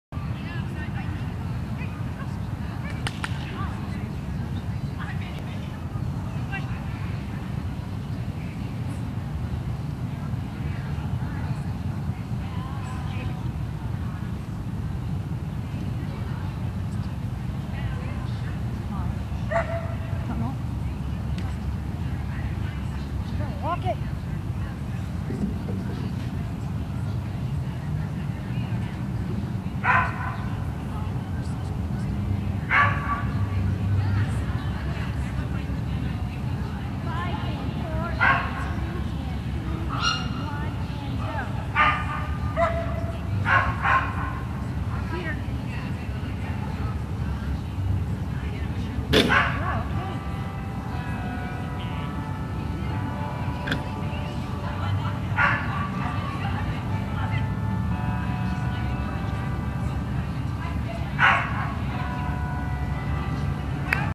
Martha and Molly Master Std 6 28 13
Martha and Molly, Parson Russell Terrier, run an AKC Master Standard course under Judge Cindy Tilly  at the Golden Retriever Club of San Diego agility trial at NTC Promenade, San Diego,  6/28/2013. Filmed by Agility Videos 4 You